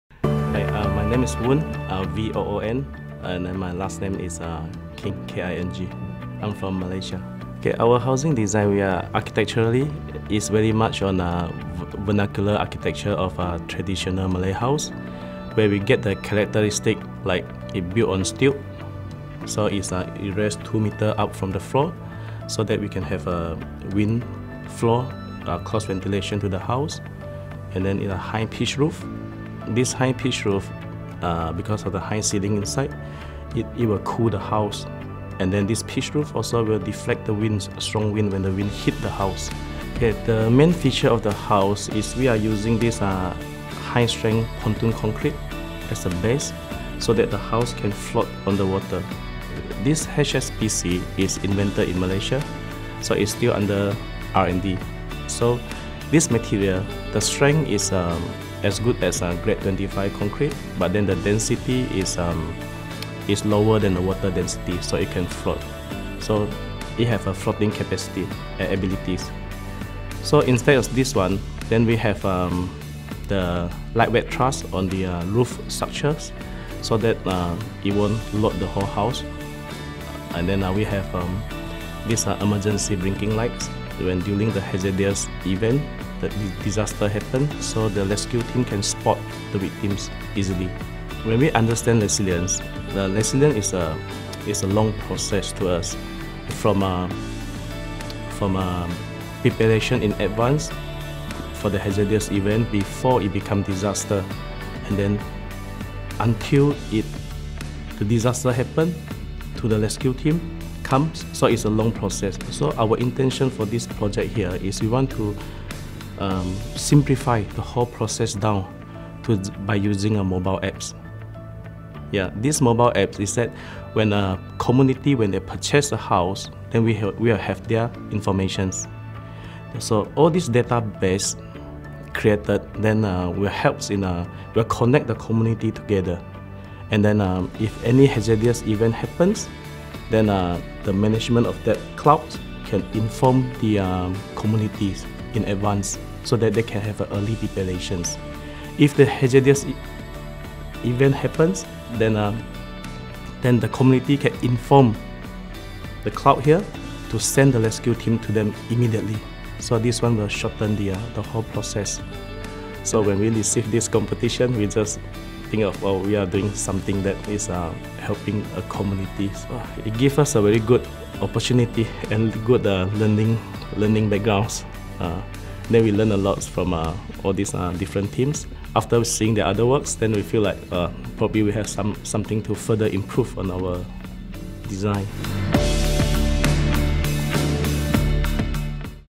Resilient Homes Design Challenge: A Resilient Home in Malaysia by Architects Avenue
Voon King from Architects Avenue talks about their winning entry to the Resilient Homes Challenge, a design for a flood-resilient home in Malaysia. The home has amphibious features, so they've called their design "Apung House" which means "floating house."